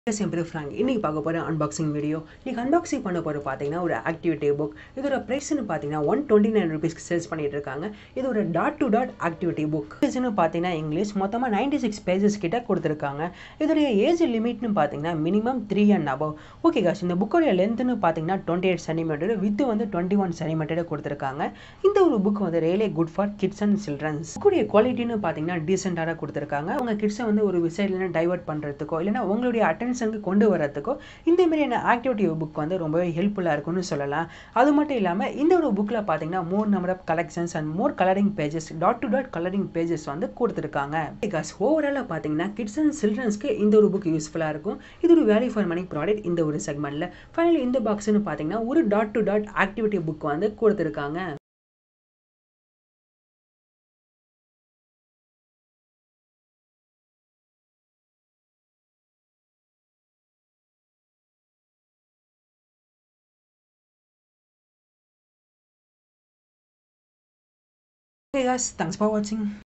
Prety Colourful 101 Dot To Dot Activity Book| Tamil Unboxing
Hi Guys,

Today I'm Going To Unbox 101 Dot To Dot Colouring.